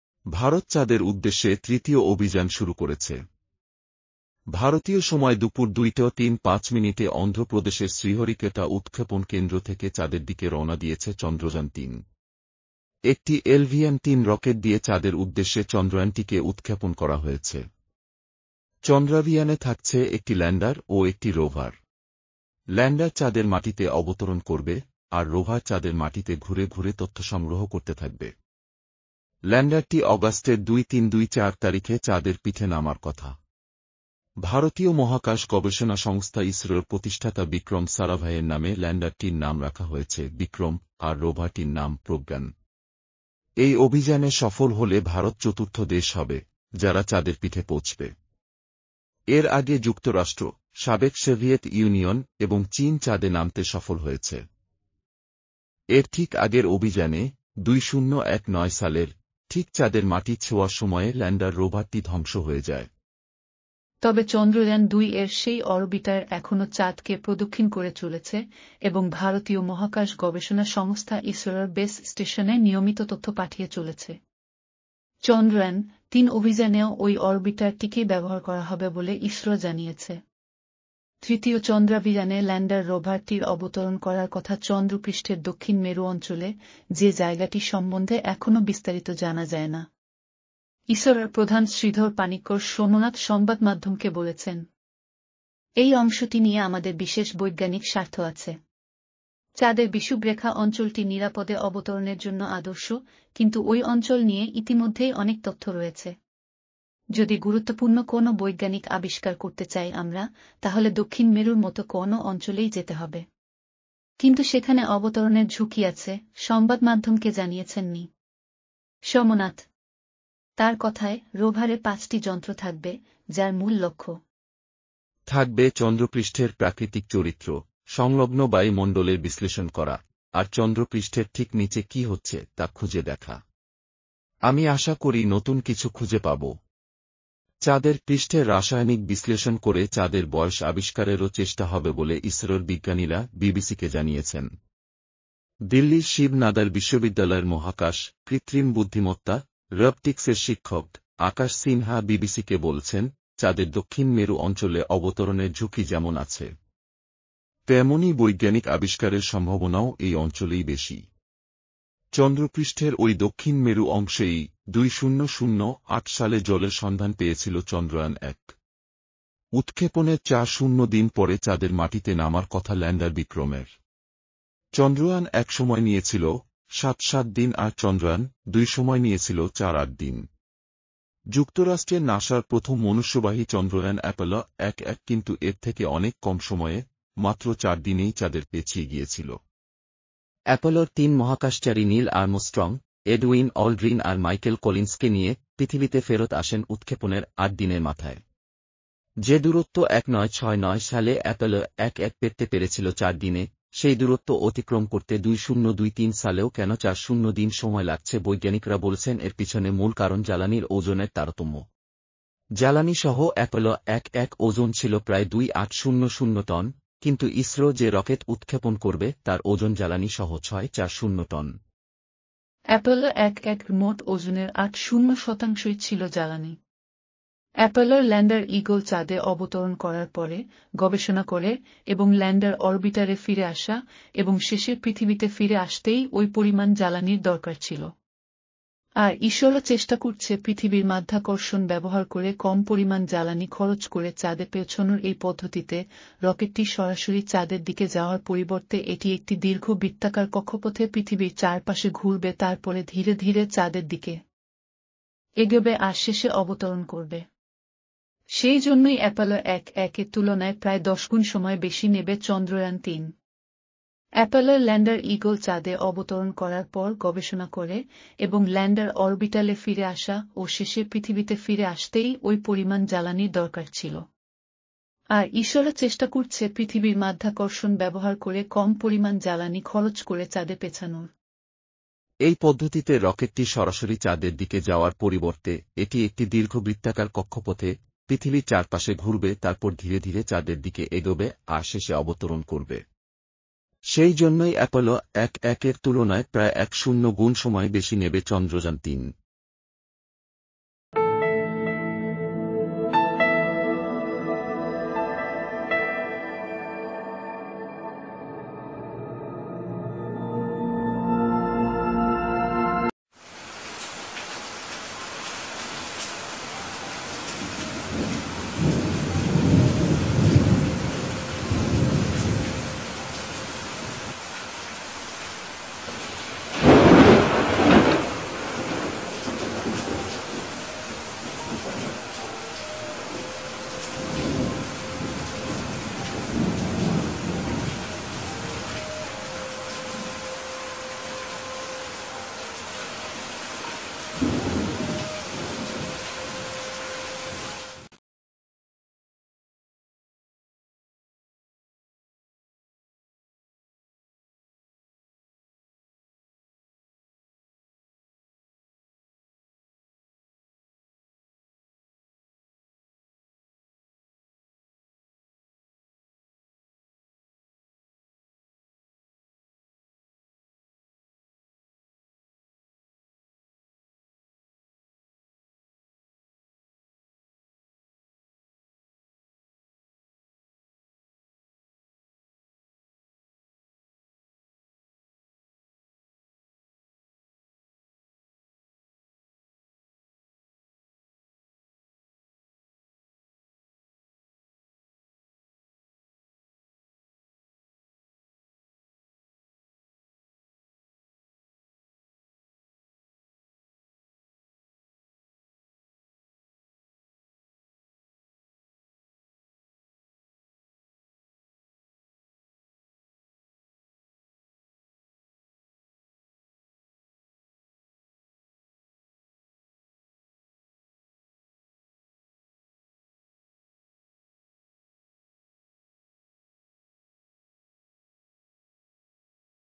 চাঁদের দেশে ছুটছে ভারতের চন্দ্রযান-৩, যাত্রা Chandrayaan-3 is running to land of the moon a journey
ভারত  চাঁদের উদ্দেশ্যে তৃতীয় অভিযান শুরু করেছে। ভারতীয় সময় দুপুর ২টো ৩৫ মিনিটে অন্ধ্র প্রদেশের শ্রীহরিকোটা উৎক্ষেপণ কেন্দ্র থেকে চাঁদের দিকে রওনা দিয়েছে চন্দ্রযান ৩। একটি এলভিএম-৩ রকেট দিয়ে চাঁদের উদ্দেশ্যে চন্দ্রযানটিকে উৎক্ষেপণ করা হয়েছে।
চন্দ্রাভিযানে থাকছে একটি ল্যাণ্ডার ও একটি রোভার। ল্যাণ্ডার চাঁদের মাটিতে অবতরণ করবে আর রোভার চাঁদের মাটিতে ঘুরে ঘুরে তথ্য সংগ্রহ করতে থাকবে।